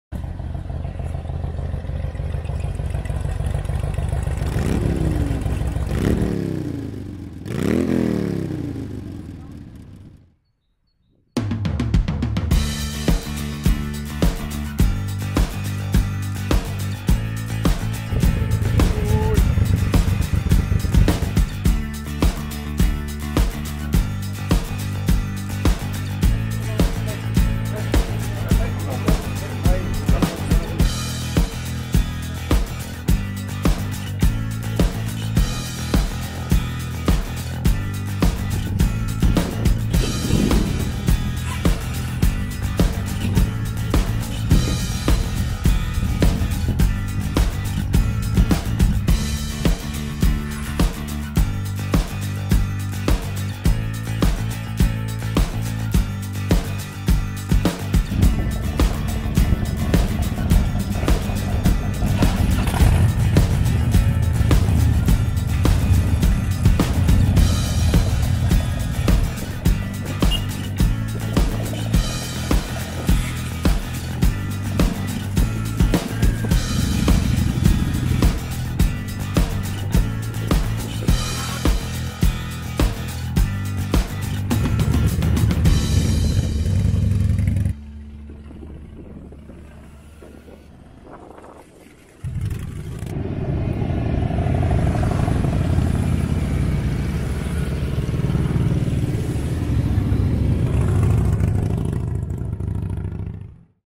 Start Bikerburger rit April 2019
Edwin als voorrijder, totaal 8 motoren, onderweg naar Bistro het Kleine  Verschil voor de enige echte Bikerburger.